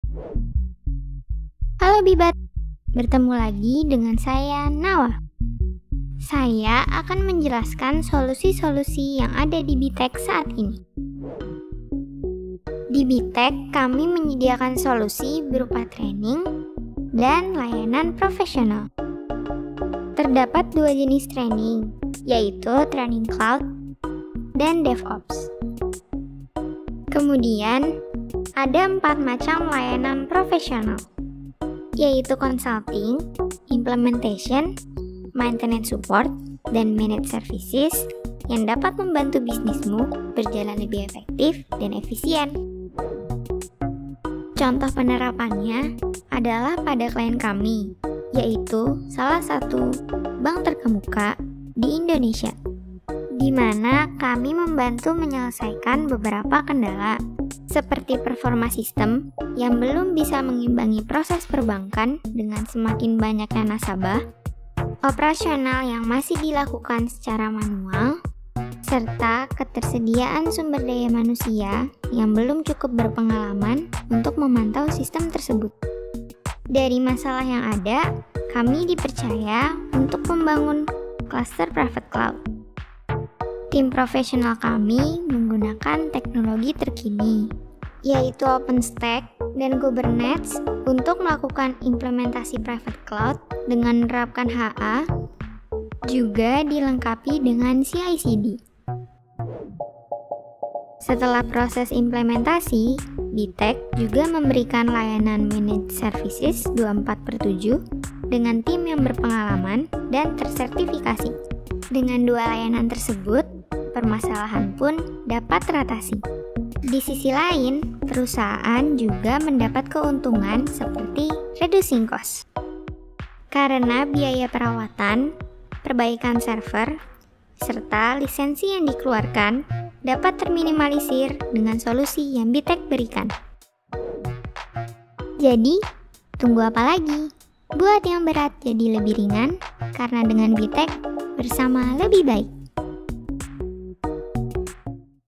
Solusi Cloud pada Industri Perbankan
Penerapan sistem Cloud yang efisien pada industri perbankan tentunya memiliki tantangan tersendiri. Oleh karena itu, langkah-langkah yang tepat perlu diperhatikan dengan baik.

Boer Technology hadir menjawab tantangan tersebut dengan solusi yang tepat. Dimulai Implementasi yang akurat, serta dukungan Managed Services oleh tim bersertifikat. Kami siap mewujudkan mimpi Anda untuk melesat. Karena kita, BersamaLebihBaik.

Simak selengkapnya tayangan berikut.